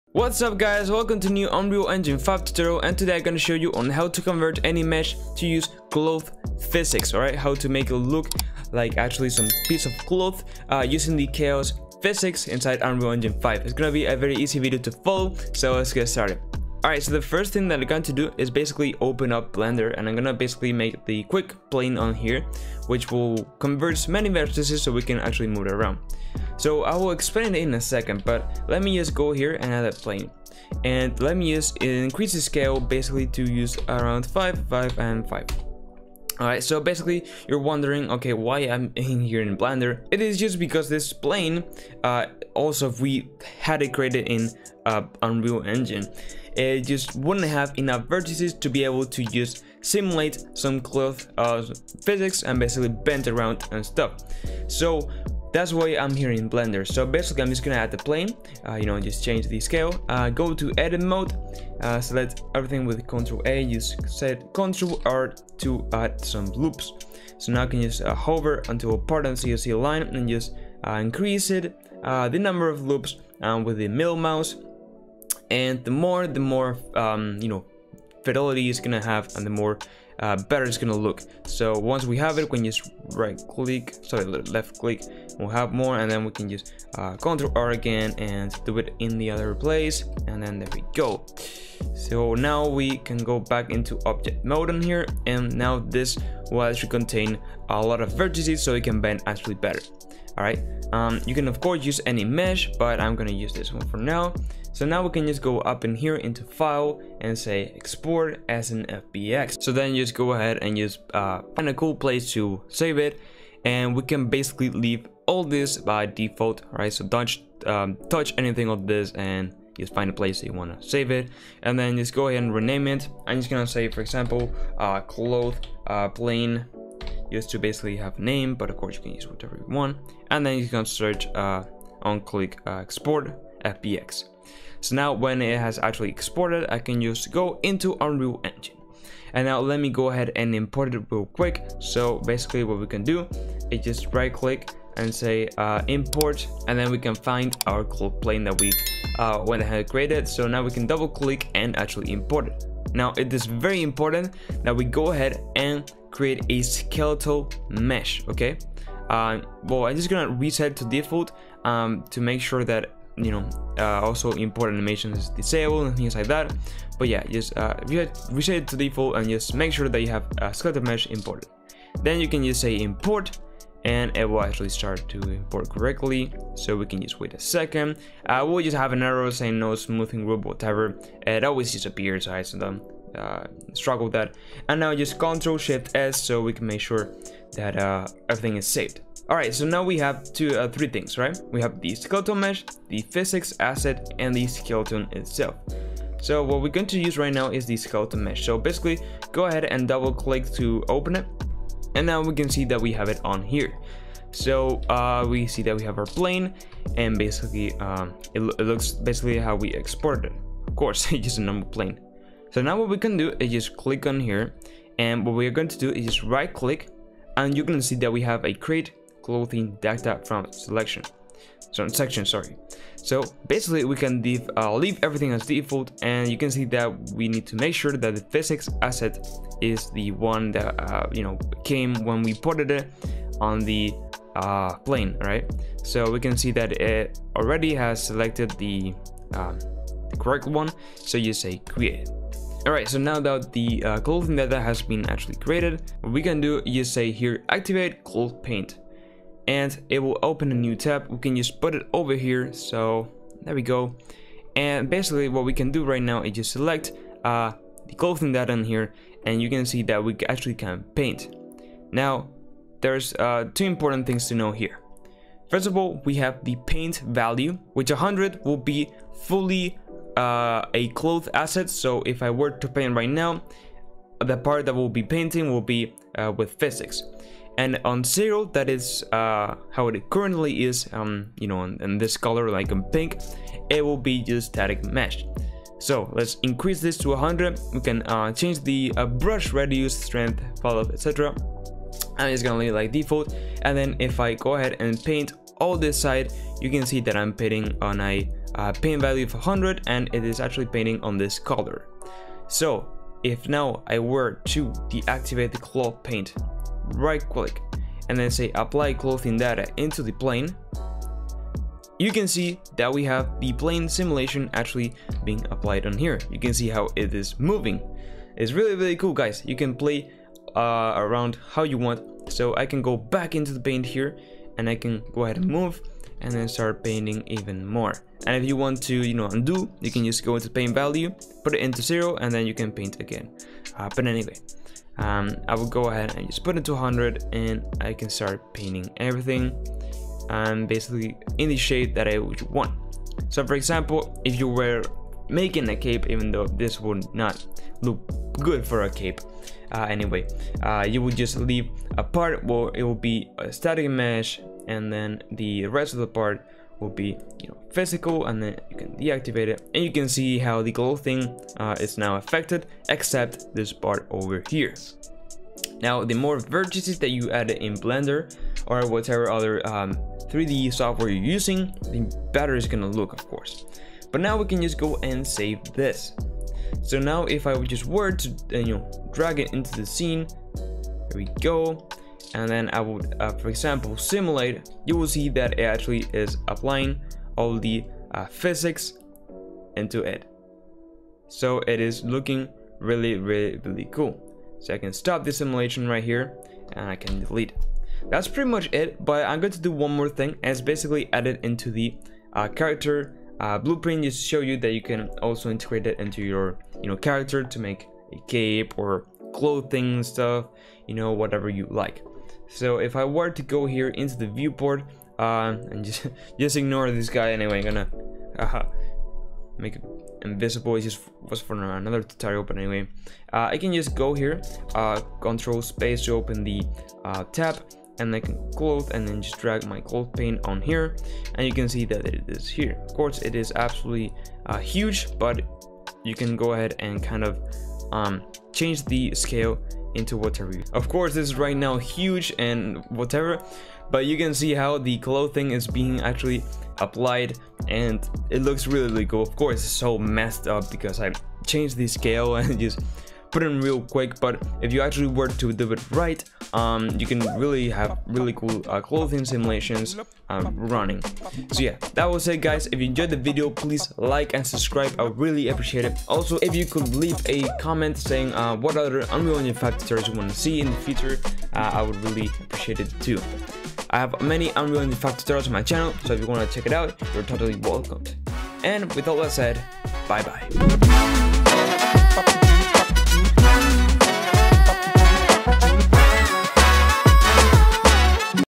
Create a Cloth Simulation in Unreal Engine 5 - A Step-By-Step Tutorial
Hello guys, in this quick and simple tutorial we are going to learn how to create a Chaos Cloth Simulation in real-time using any mesh in Unreal Engine 5.